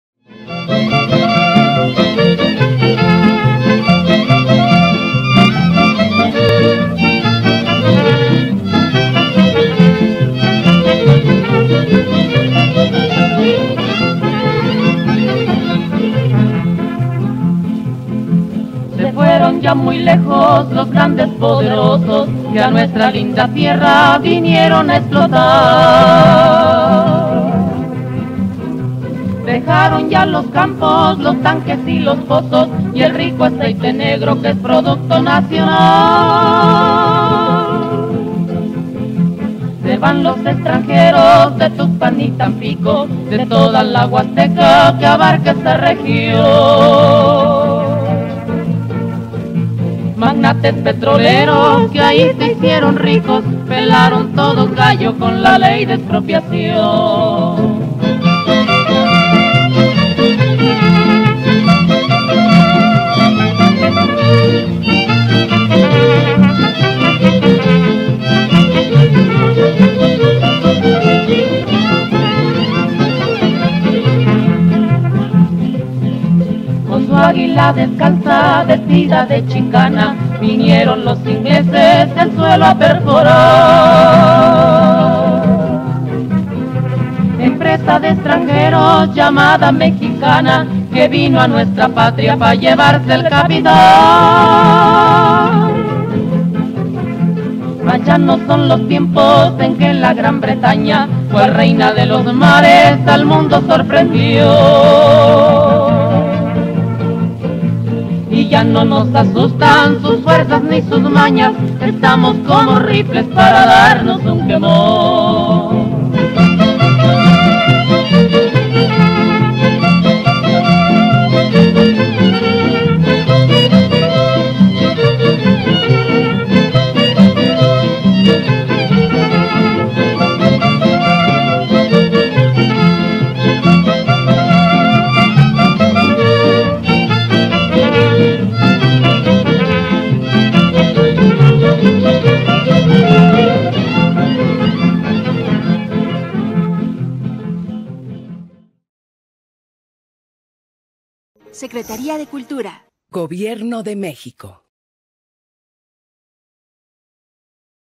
La Expropiación petrolera
Te invitamos a conocer sobre la expropiación petrolera y lo que significó para el país en esta cápsula sobre uno de los momentos más importantes del gobierno de Lázaro Cárdenas.